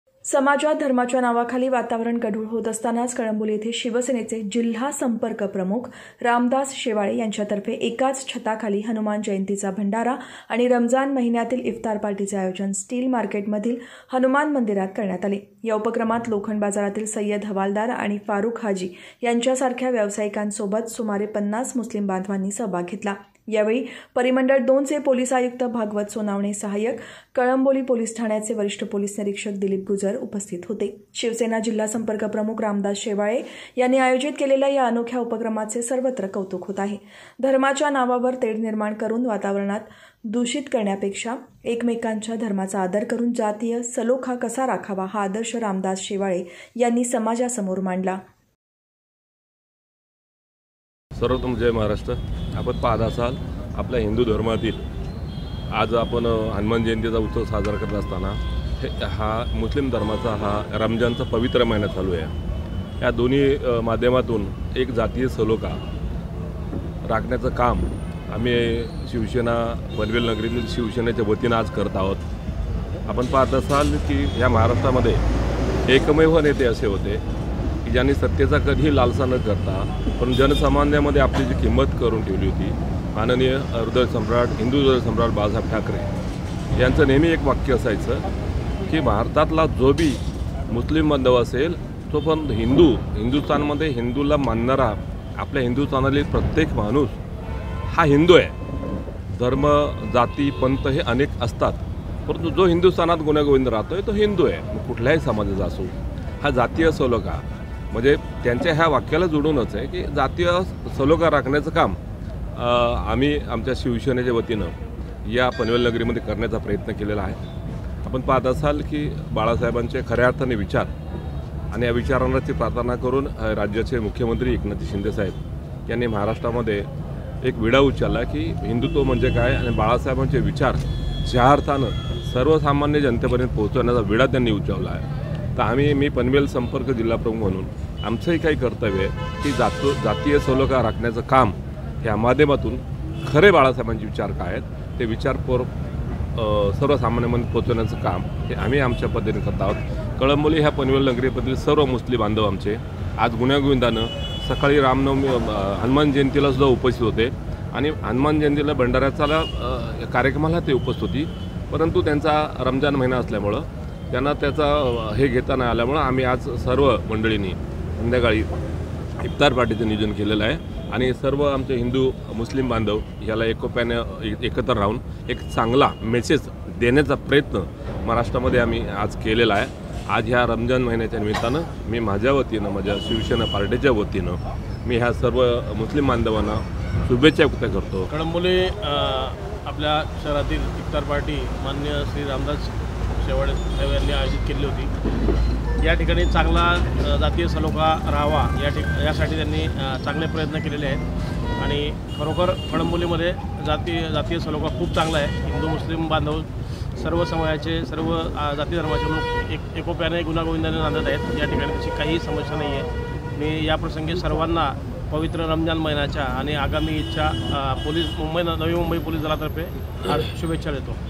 कळंबोली येथील हनुमान जयंती मंडपात ईफ्तार पार्टी हिंदू मुस्लीम ऐक्याचे प्रतिक !
समाजात धार्माच्या नावाखाली वातावरण गढूळ होत असतानाच कळंबोली येथे शिवसेना जिल्हासंपर्कप्रमुख रामदास शेवाळे यांच्या तर्फे एकाच छताखाली हनुमान जयंतीचा भंडारा व रमजान महिन्यातील इफ्तार पार्टीचे आयोजन कळंबोली वसाहतीमधील स्टील मार्केट मधील हनुमान मंदिरात करण्यात आले.या उपक्रमात लोखंड बाजारातील सय्यद हवालदार व फारुक हाजी यांच्यासारख्या व्यावसियांकासोबत सुमारे 50 मुस्लिम बांधवांनी सहभाग घेतला.